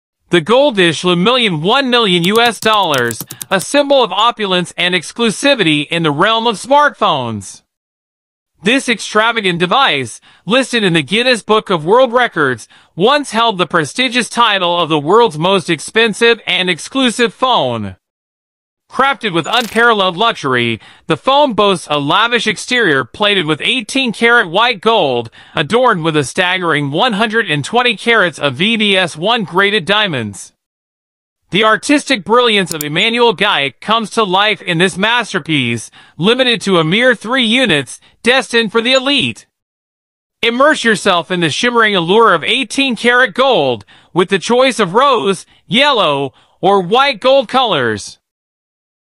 "GoldVish Revolution: The $1 Million Smartphone Unveiled!"
Welcome to the epitome of luxury tech – the GoldVish Revolution! In this exclusive video, we delve into the opulent world of the $1 Million smartphone, exploring its unparalleled craftsmanship, extravagant features, and the sheer indulgence it offers. Join us as we unbox this symbol of status, discuss its cutting-edge technology, and witness the fusion of art and innovation. If you've ever wondered what it's like to own a phone that redefines luxury, this is your front-row seat. Get ready to experience the epitome of sophistication in the palm of your hand with the GoldVish Revolution. Luxury has a new name!